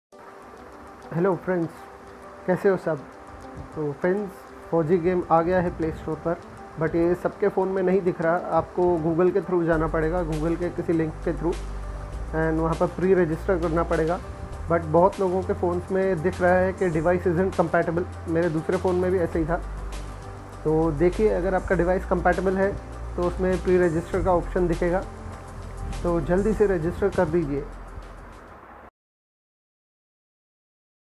FAUG game registration started on playstore | Dekhiye ye video me maine kaise register kiya.
FAUG game playstore par aagya hai.Lekinsabke phones par search me nahi dikh raha & bahot logon ke phones me device is'nt compatible aaraha hai.
Check kariye ( Agar aapka phone compatible hai to jaldi register kardijiy )

FAUG game par registration karne ke liye neeche deegayee link par click karo.